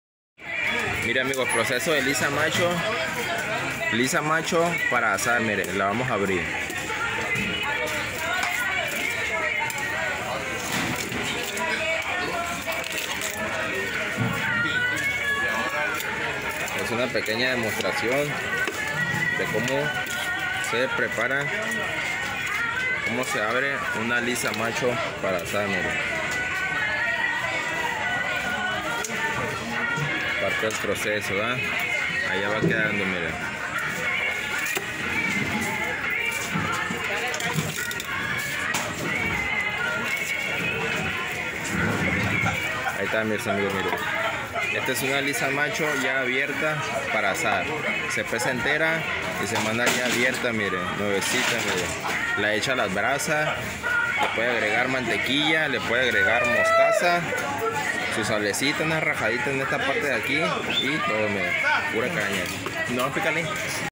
abriendo una lisa macho para azar
proceso de abrir una Lisa macho para asar Cómo se abre para que ya no batalle